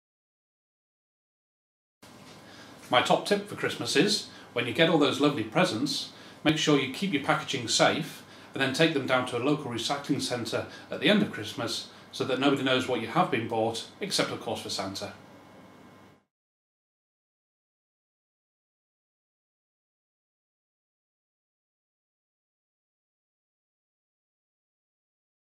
Christmas Advent Calendar 2012 - Day 24 - PCSO Dan Sillers
PCSO Dan Sillers from Wetherby NPT shares his top tip for staying safe this Christmas.